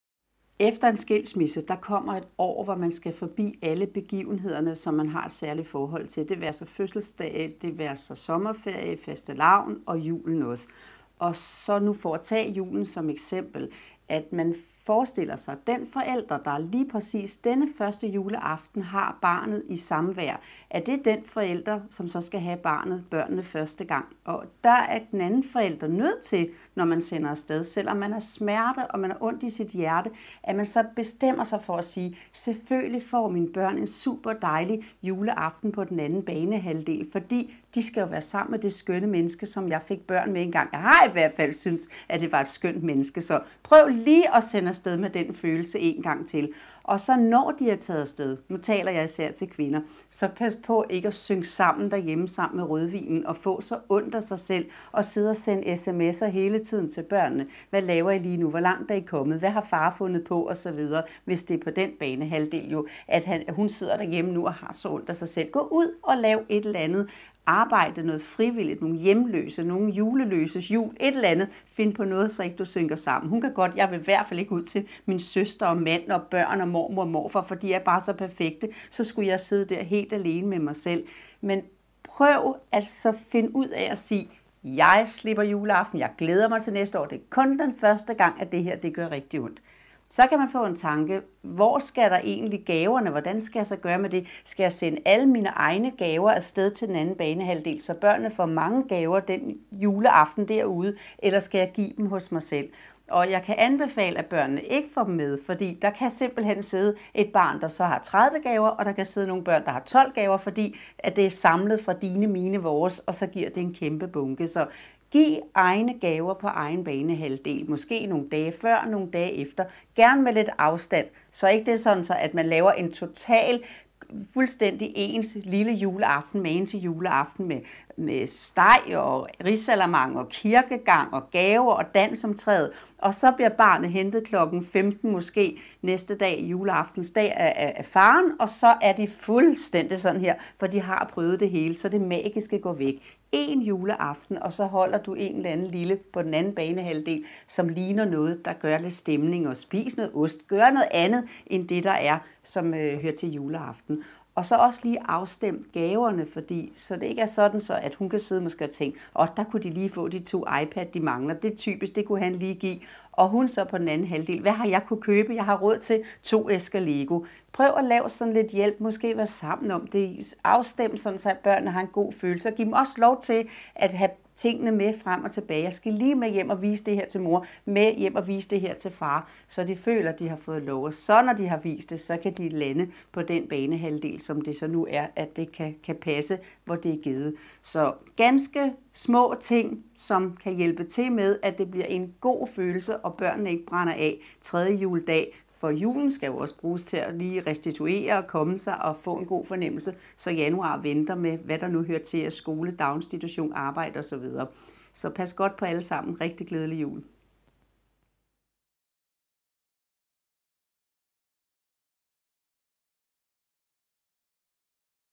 Hvordan giver skilsmisseforældre børnene den bedste jul?
Lola Jensen, familievejleder og forfatter til bogen "Pas på familien, også når den går i stykker."